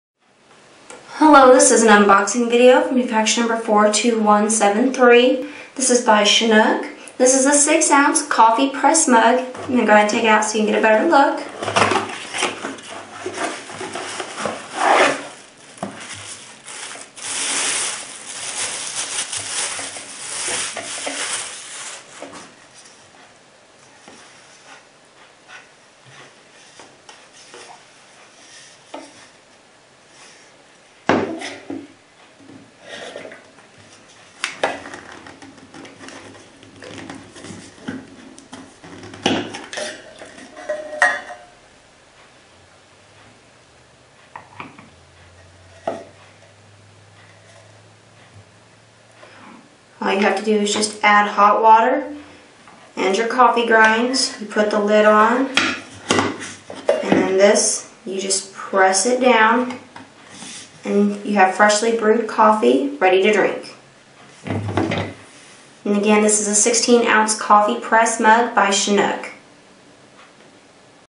Chinook Coffee Press Mug 16oz 42173 Unboxing
This is our unboxing video for the Coffee Press Mug 16oz mfg# 42173. At Wholesale Hunter